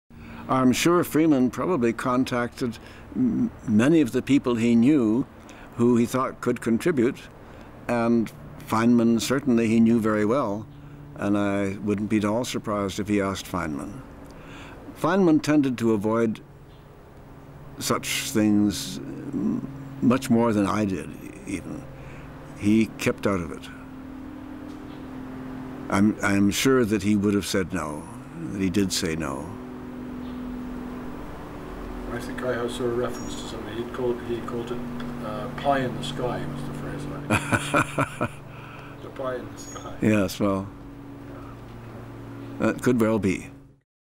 Robert Christy - Richard Feynman rejected Project Orion (17/20)
Robert Christy (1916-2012) was an American theoretical physicist who worked on the Manhattan Project at Los Alamos to develop the first atomic bomb during World War II. Also a member of the unsuccessful Project Orion, Christy was a former president of Caltech. Listener: Christopher Sykes